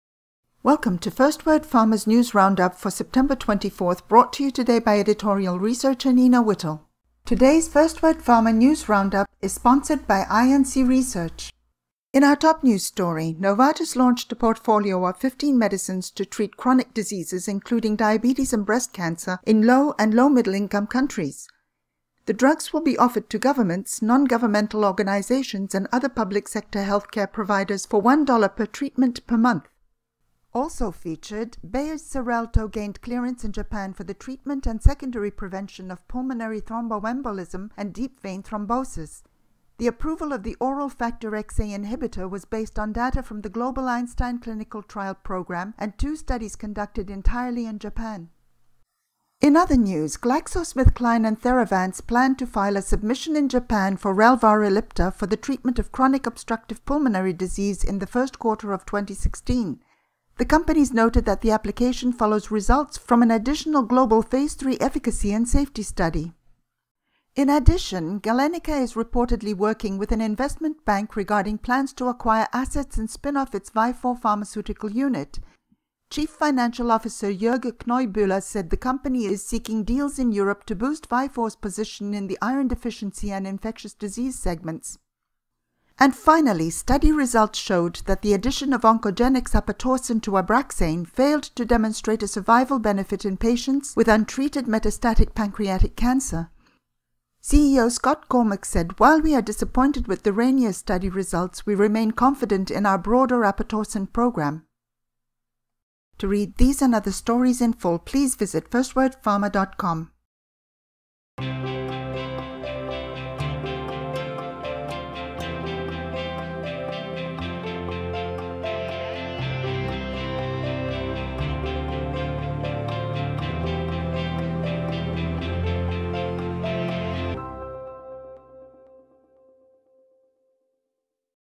FirstWord Pharma Daily News Round-Up Video for September 24, 2015
The FirstWord Pharma Daily News Round-Up Video gives a recap of the latest pharmaceutical and biotechnology news. Stay abreast of the key industry news each day. Today's video highlights include Novartis, Bayer, Xarelto, GlaxoSmithKline, Theravance, Relvar Ellipta, Galenica, Vifor, OncoGenex, apatorsen and Abraxane.